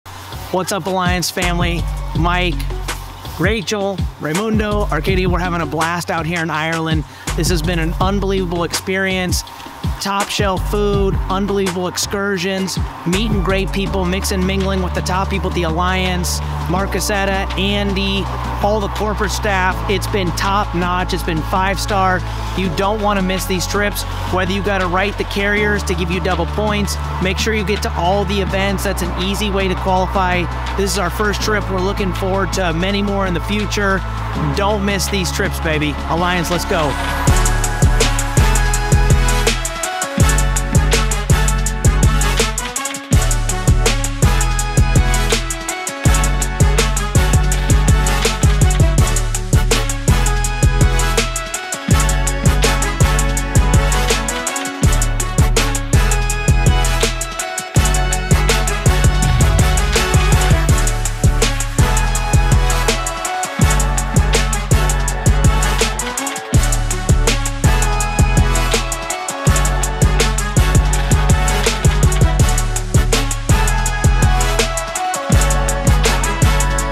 LIVE in Ireland: We Want YOU to be on Our Next Trip! | The Alliance
Join the Alliance Family on their exhilarating trip to Ireland, filled with top-notch food, unbelievable excursions, and encounters with influential leaders. The team emphasizes the importance of attending these transformative trips, whether for double points or to qualify for future events. With their first trip being a tremendous success, they express excitement for the many more adventures to come!